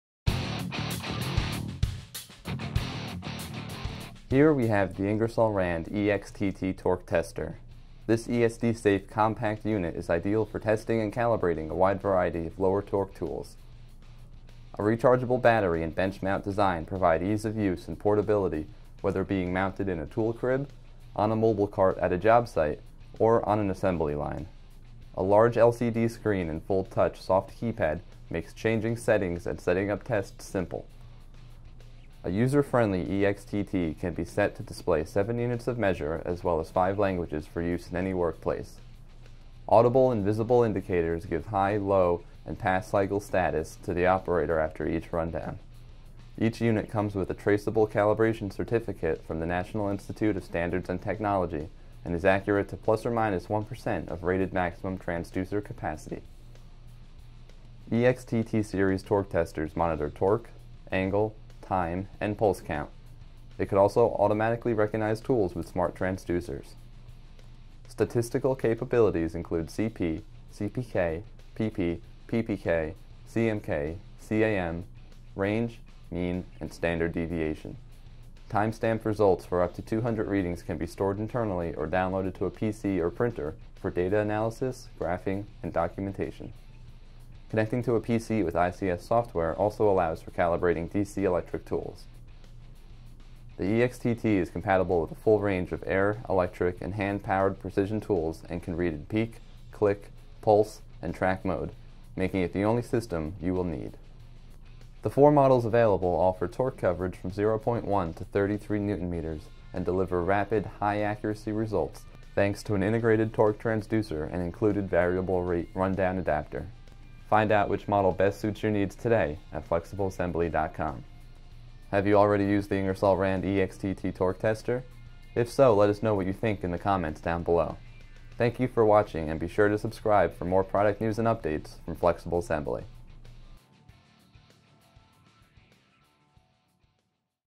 Ingersoll Rand EXTT Torque Tester
Here we have the Ingersoll Rand EXTT torque tester. This ESD-safe compact unit is ideal for testing and calibrating a wide variety of lower torque assembly tools. A rechargeable battery and bench-mount design provide ease of use and portability whether being mounted in a tool crib, on a mobile cart at a job site or on an assembly line. A large LCD screen and full soft-touch keypad makes changing settings and setting up tests simple.

The user-Friendly EXTT can be set to display 7 units of measure as well as 5 languages for use in any workplace. Audible and visible indicators give high, low, and pass cycle status to the operator after each rundown. Each unit comes with a traceable calibration certificate from the National Institute of Standards and Technology and is accurate to +/-1% of rated maximum transducer capacity.

EXTT series torque testers monitor torque, angle, time and pulse count. It can also automatically recognize tools with smart transducers. Statistical capabilities include Cp, Cpk, Pp, Ppk, CMK, CAM, range, mean and standard deviation. Time-stamped results for up to 200 readings can be stored internally and downloaded to a PC or printer for data analysis, graphing and documentation. Connecting to a PC with ICS software allows for calibrating DC electric tools.

The EXTT is compatible with full range of Air, Electric, and Hand powered precision tools and can read in peak, click, pulse, or track mode making it the only system you will need. The four models available offer torque coverage from 0.1 to 30 Nm and deliver rapid high-accuracy results thanks to an integrated torque transducer and included variable rate rundown adapter.

Products used in this video:
- EXTT-30 Torque Tester
- QXX2AT27PS6 Right Angle Nut Runner